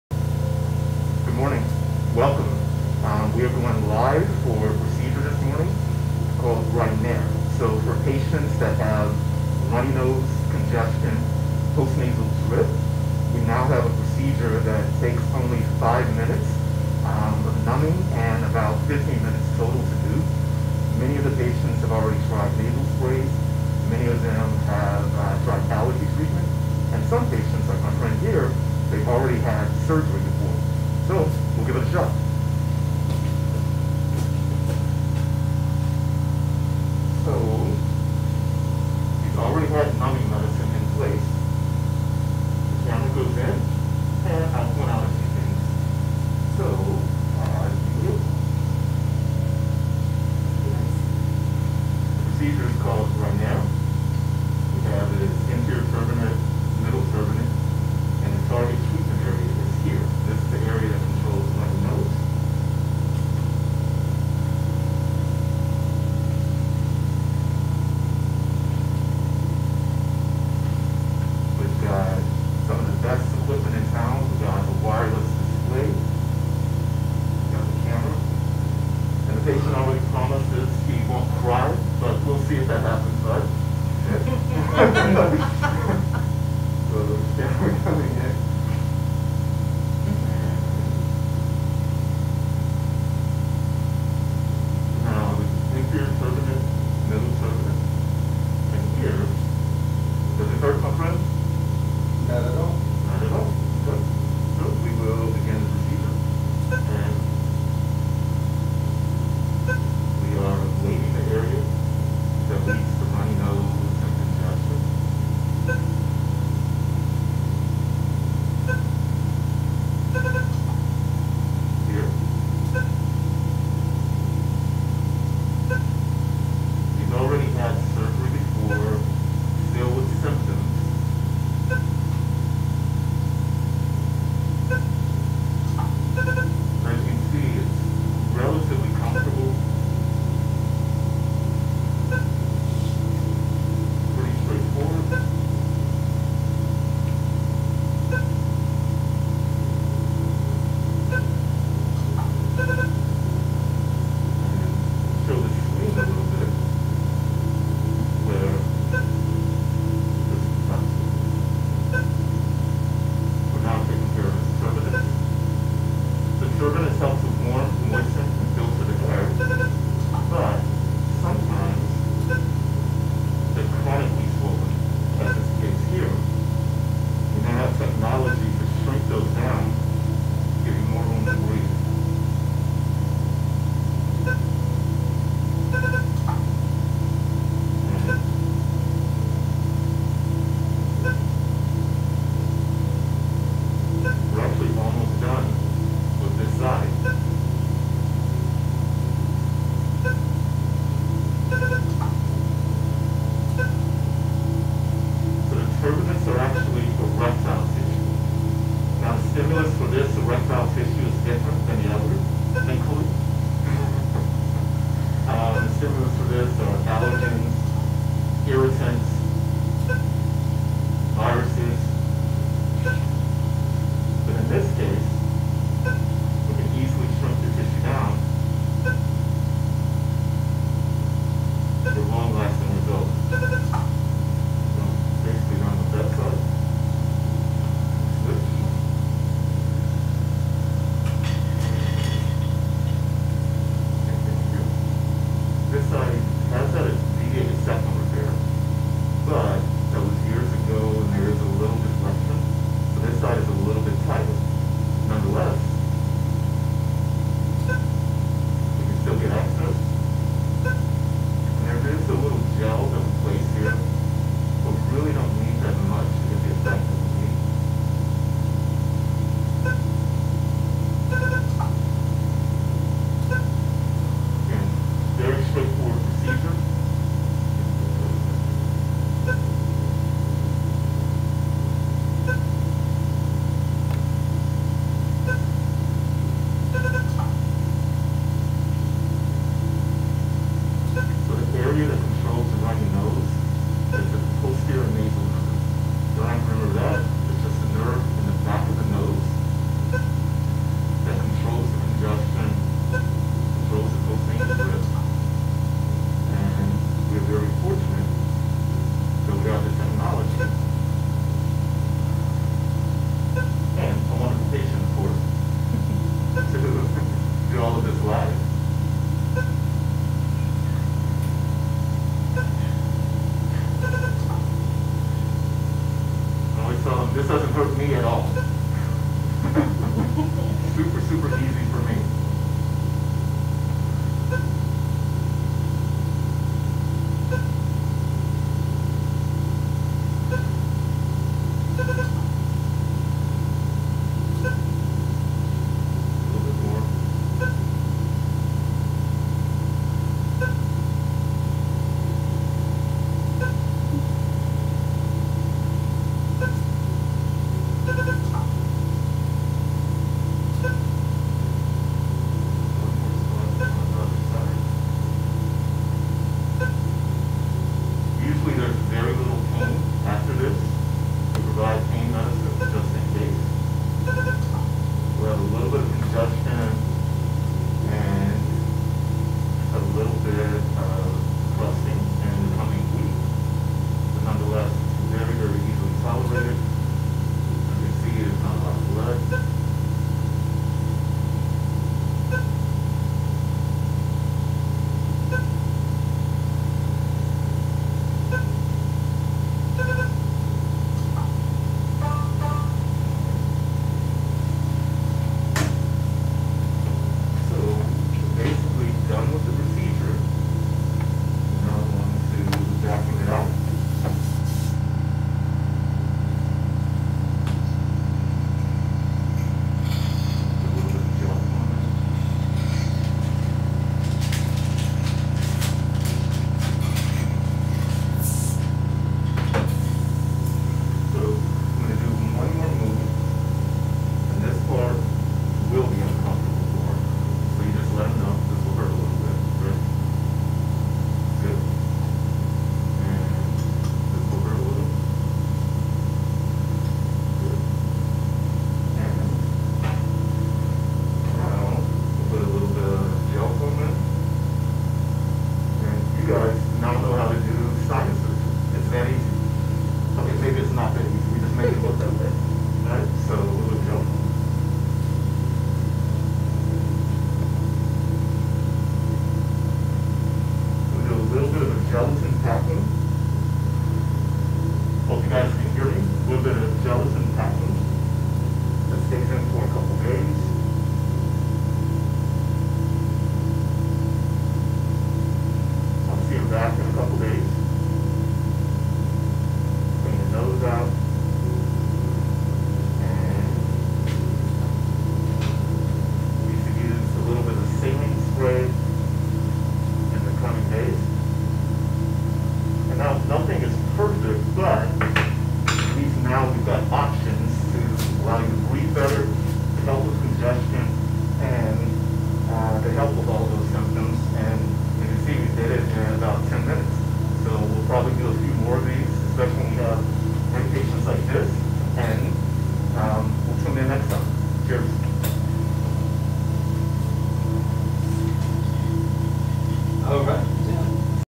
Rhinaer and Vivaer (minimally invasive options to improve breathing)
Rhinaer is a minimally invasive procedure that helps with
runny nose, congestion, and post nasal drip.

Live in office surgery